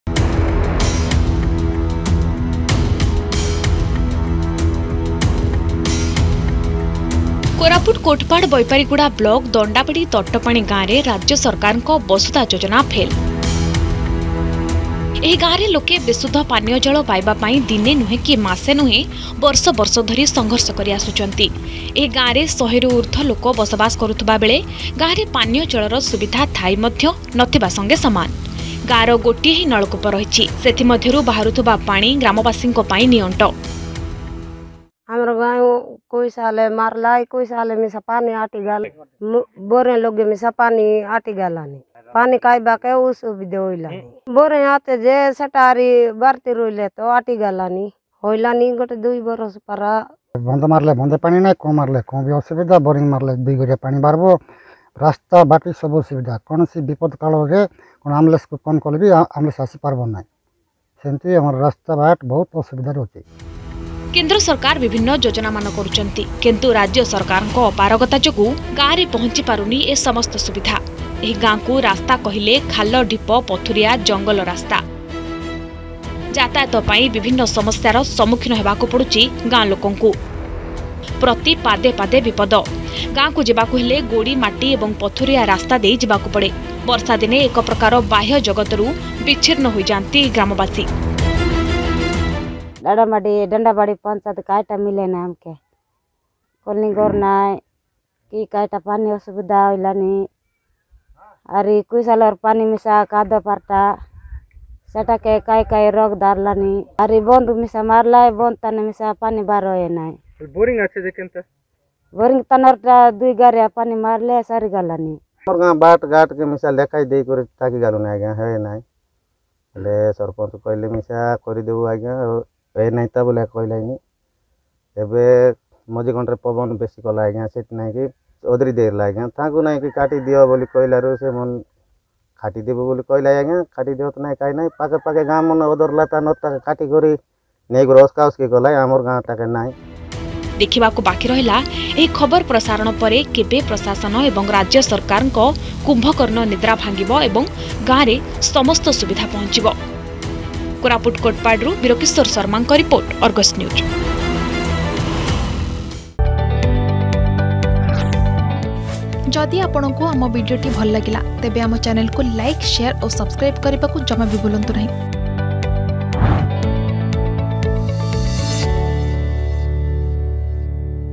୧୦୦ ରୁ ଉଦ୍ଧ୍ବ ଲୋକଙ୍କ ପାଇଁ ଚୁଆଁ ପାଣି ସାହା ଭରଷା
କୋରାପୁଟ କୋଟପାଡ଼ ବୈପାରୀଗୁଡା ବ୍ଲକ ଦଣ୍ଡାବାଡି ତଟାପାଣି ଗ୍ରାମରେ ରାଜ୍ୟ ସରକାରଙ୍କ ବସୁଧା ଯୋଜନା ଫେଲ,ଏହି ଗ୍ରାମରେ ଲୋକେ ବିଶୁଦ୍ଧ ପାନୀୟ ଜଳ ପିଇବା ପାଇଁ ଦିନେ ନୁହେଁ କି ମାସେ ନୁହେଁ ବର୍ଷ ବର୍ଷ ଧରି ସଂଘର୍ଷ କରି ଆସୁଛନ୍ତି ।

୧୦୦ ରୁ ଉଦ୍ଧ୍ବ ଲୋକଙ୍କ ପାଇଁ ଚୁଆଁ ପାଣି ସାହା ଭରଷା